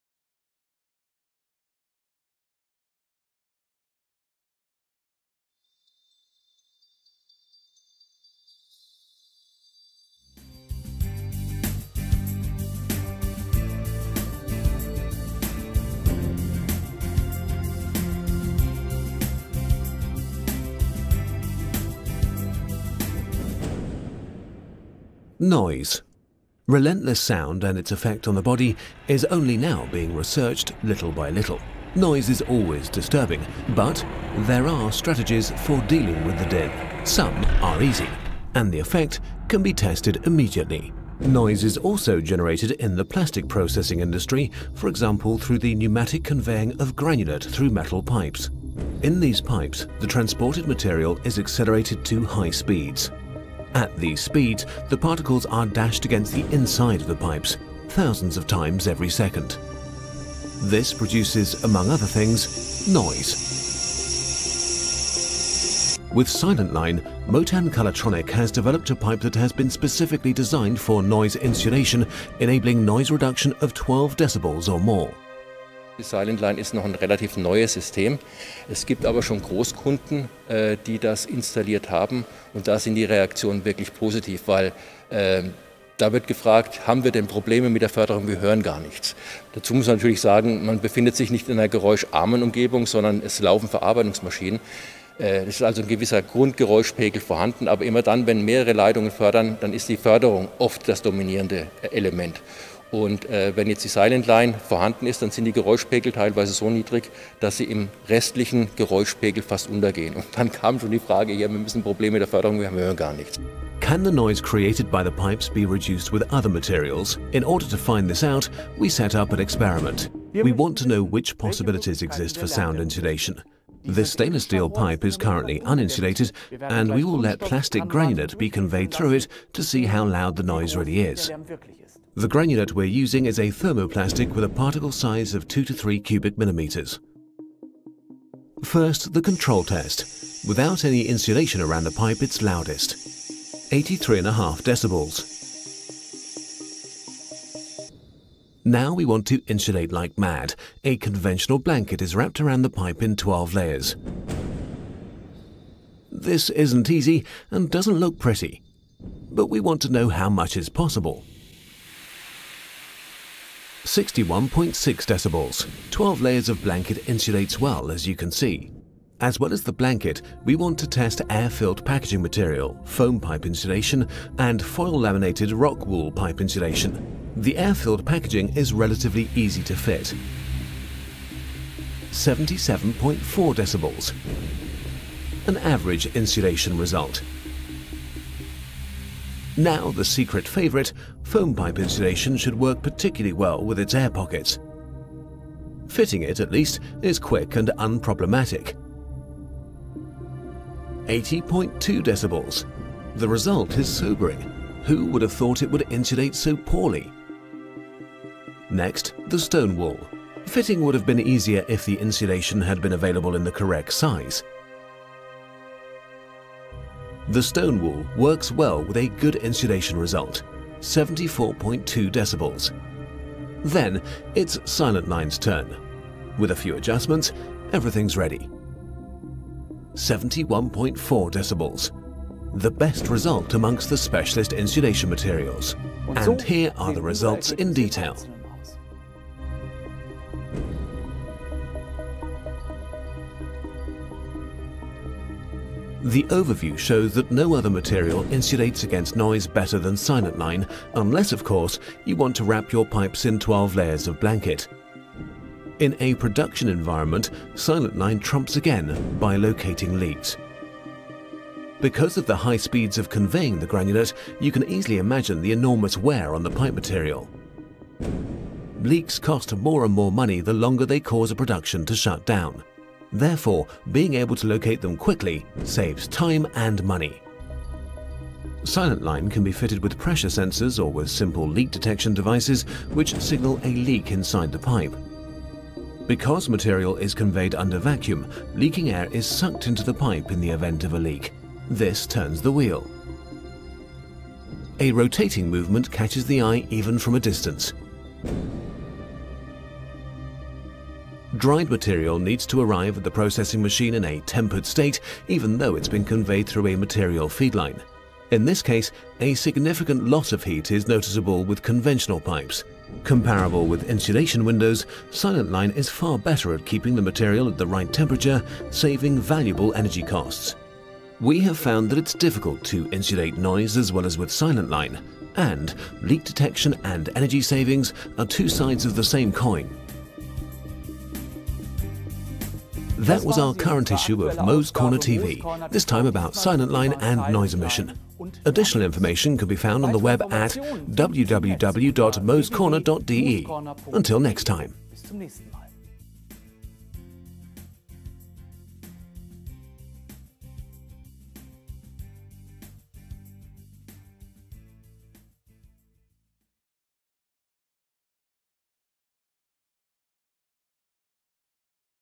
How can noise emissions during material handling be reduced? | mo's corner tv – episode 1
mo explains the methods for quiet conveying of plastic granulate
The noise levels during plastic processing are caused by many different sources. In regards to material handling there are two main sources of noise: the vacuum generator and the material feed lines.

Are you interested in more questions regarding plastics manufacturing and processing?